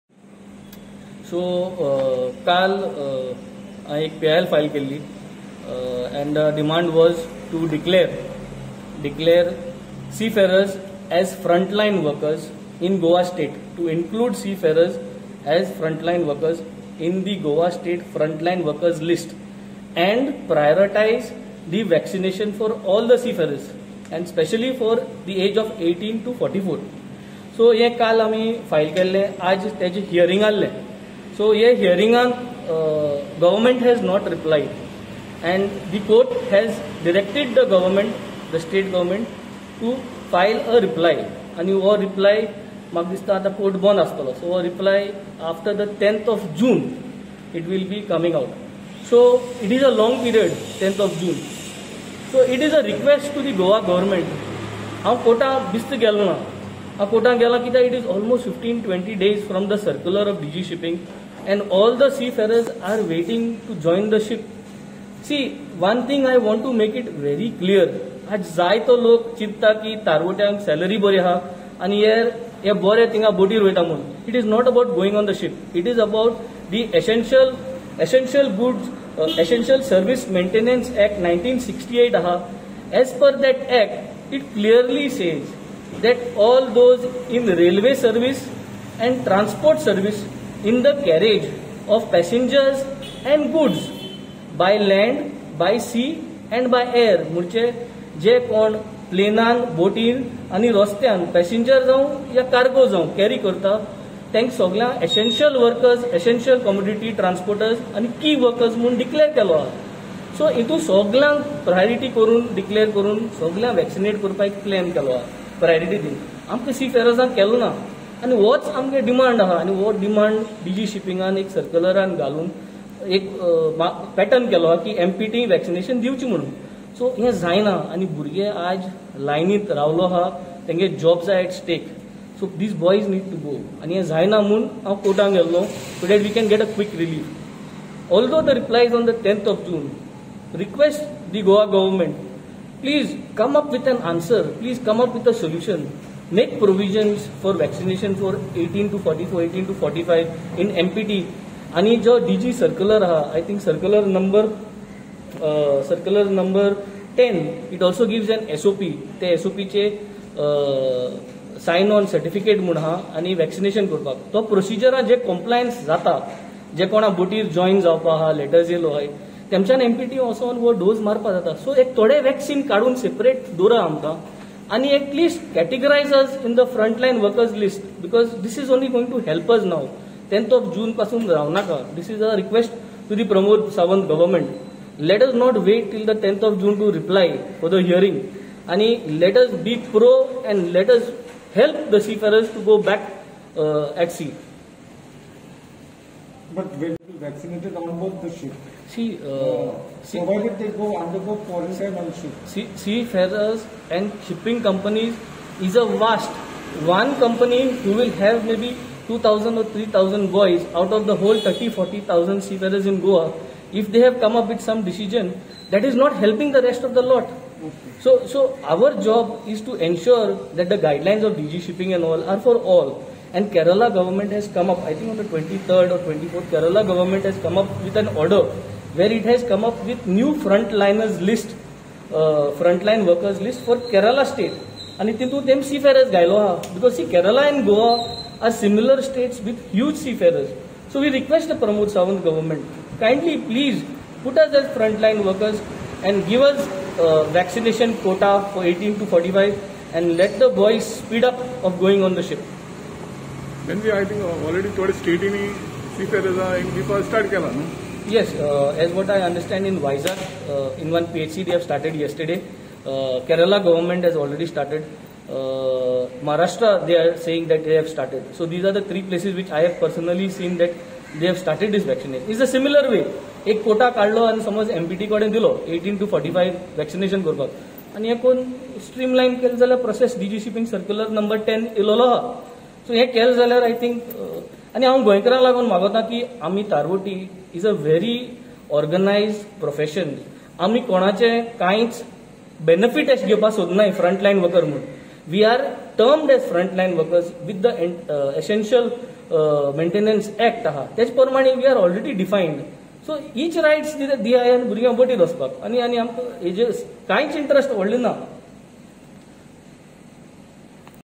PIL HAS BEEN FILLED TO DECLARE GOAN SEAFARERS AS FRONTLINE WORKERS ON PRIORITY VACCINATION
Video from GOAN REPORTER